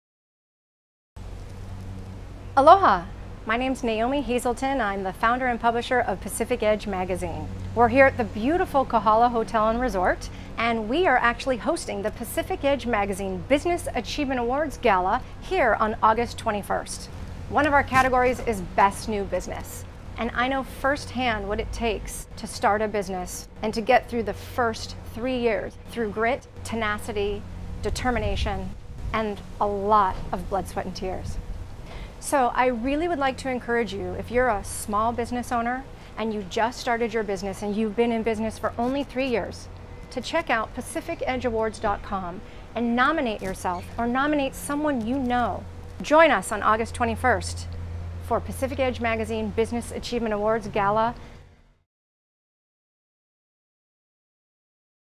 New Business- Business Achievement Awards
Pacific Edge Magazine introduces the 2019 Business Achievement Awards category, Best New Business.